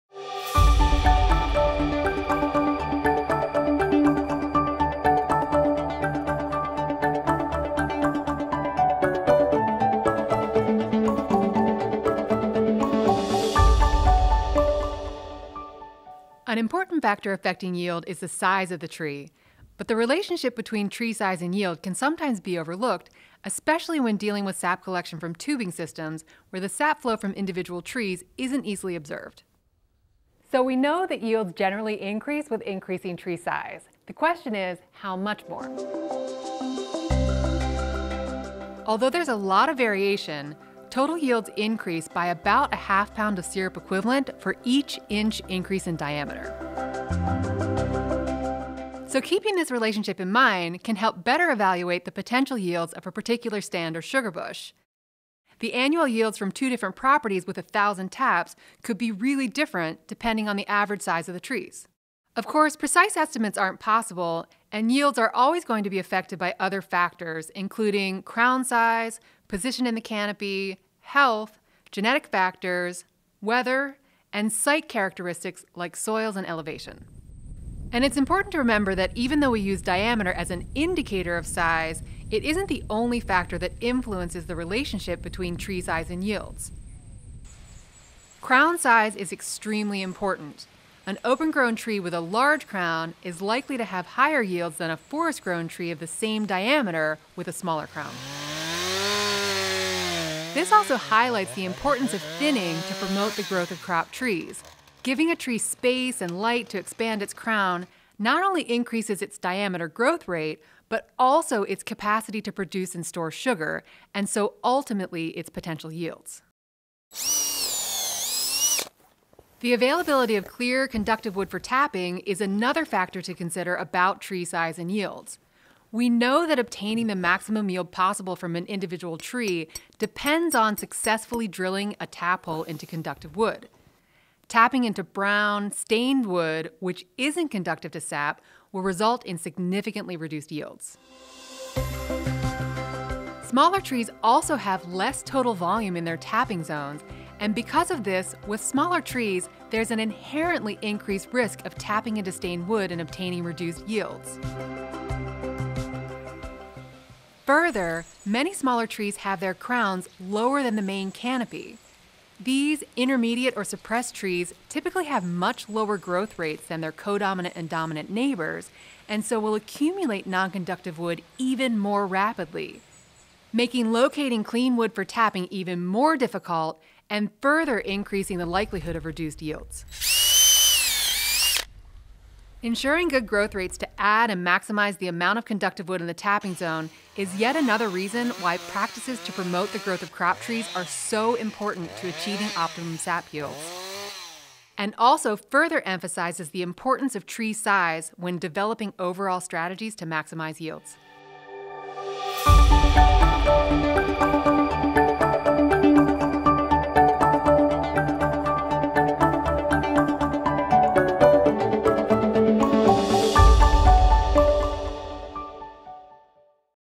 Tree Size and Yields - Keys to High Maple Sap Yields
The relationship between tree size (diameter), crown size, and sap yield from maple trees.